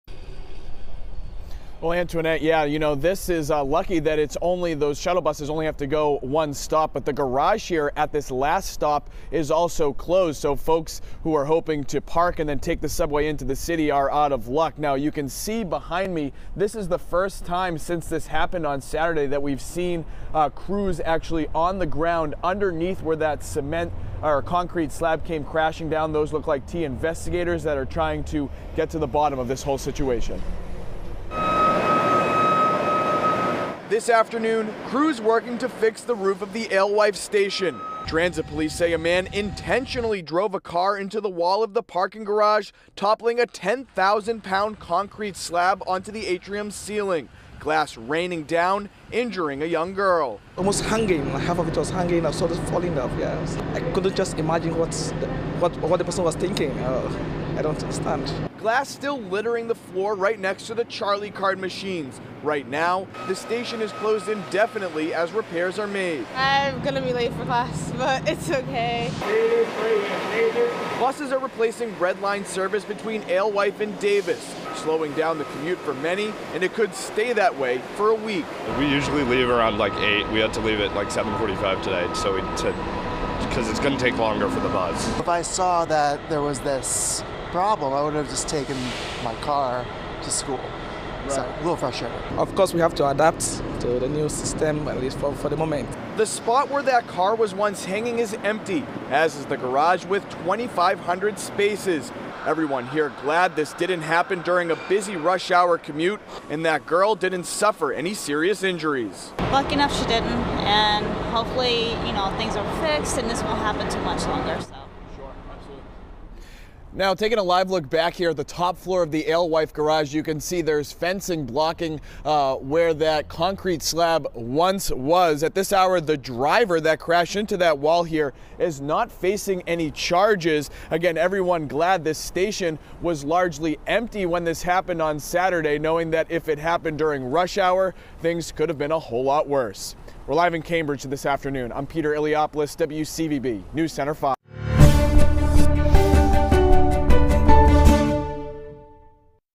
Shuttle buses in use after crash at MBTA station
The Alewife MBTA Station is indefinitely closed after a car crashed into a barrier on the top deck of the Alewife parking garage, with the debris causing an injury to a girl in the lobby of the station below, according to authorities.